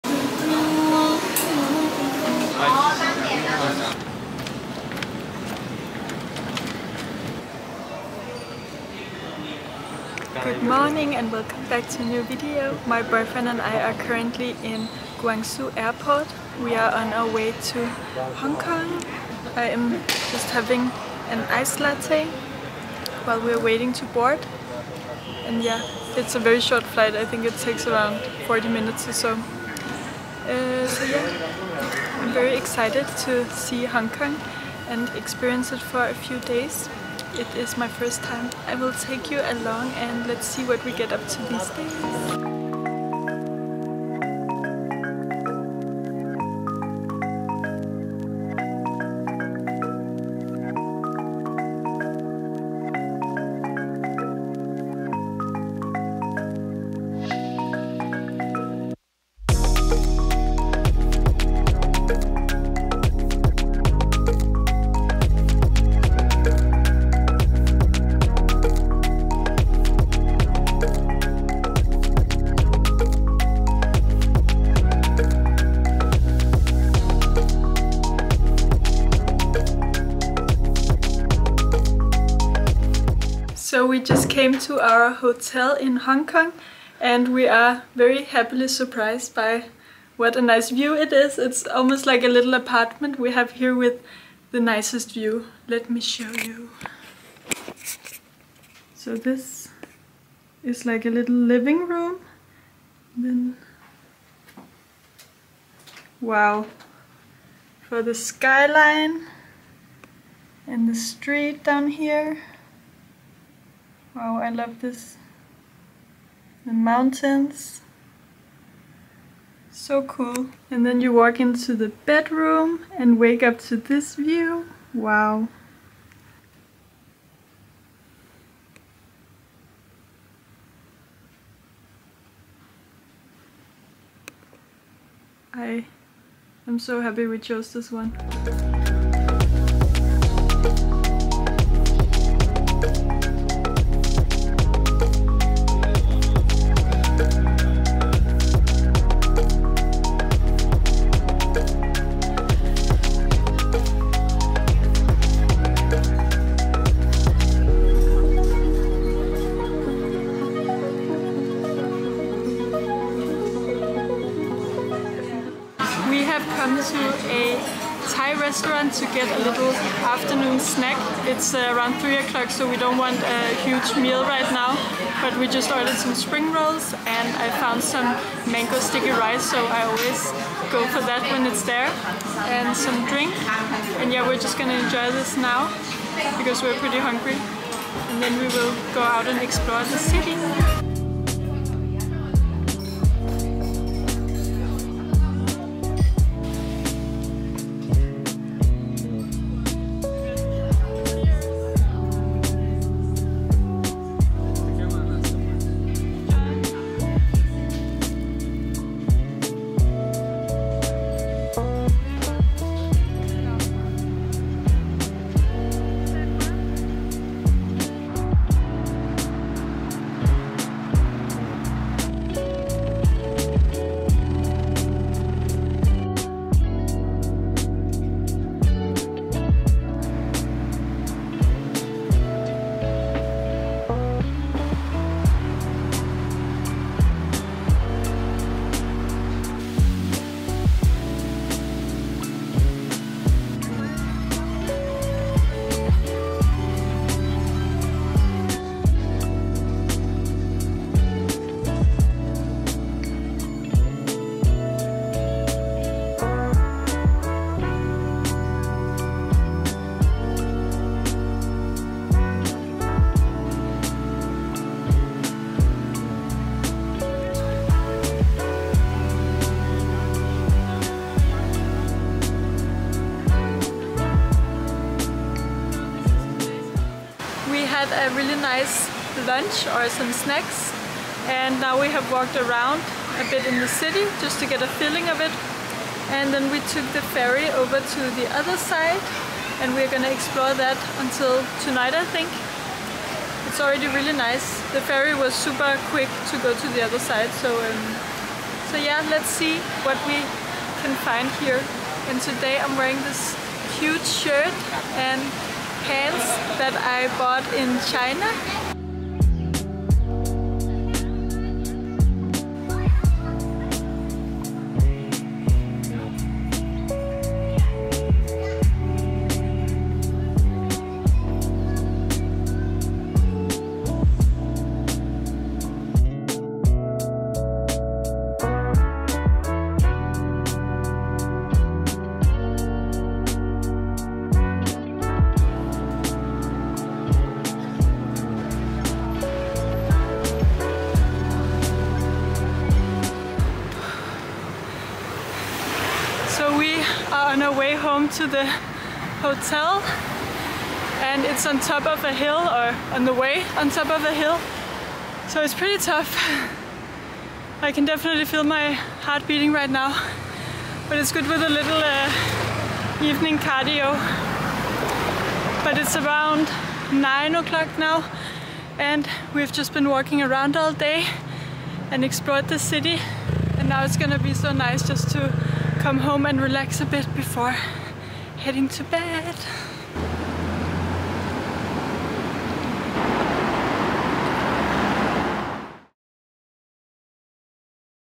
Traveling to Hong Kong: Stunning Views and Delicious Mango Sticky Rice🥭
Join me on an unforgettable journey in my latest video, "Traveling to Hong Kong: Stunning Views and Delicious Mango Sticky Rice🥭." Explore the dynamic city landscape and breathtaking skyline views. Have you ever visited Hong Kong? Share your experiences in the comments below!

From the moment I arrived in Hong Kong, I was captivated by its vibrant energy and endless opportunities for exploration. In this video, join me as I embark on an exciting journey from Guangzhou to Hong Kong. After a short flight, we'll check into our hotel and set out to discover the city's many wonders.

Our adventure kicks off with a stroll through Hong Kong's bustling streets, where the city's rich culture and dynamic atmosphere come alive. We’ll indulge in the city's culinary delights, starting with the heavenly mango sticky rice – a must-try for any food lover. As the day progresses, we'll savor a delicious Indian vegan dinner, showcasing the diverse and flavorful cuisine Hong Kong has to offer.

Don't miss out on this incredible journey! Let's explore the magic of Hong Kong together!

Embark on a virtual journey through the streets of Hong Kong. Watch "Traveling to Hong Kong: Stunning Views and Delicious Mango Sticky Rice🥭" now and let the beauty and vibrancy of the city inspire you!